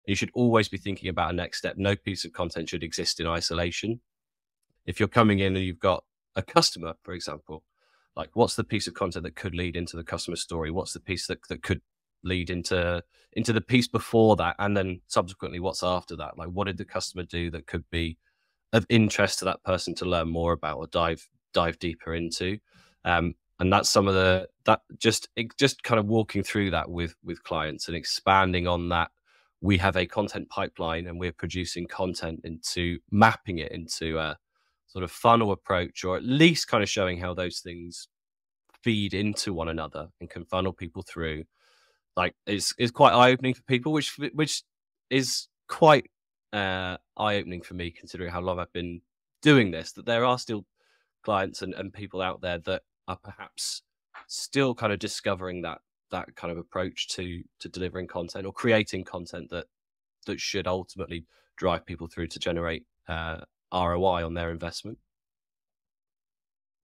The Importance of Next Steps in Content Strategy
Every piece of content should lead somewhere. This segment explores how to create content that not only engages but also guides your audience to the next step in their journey.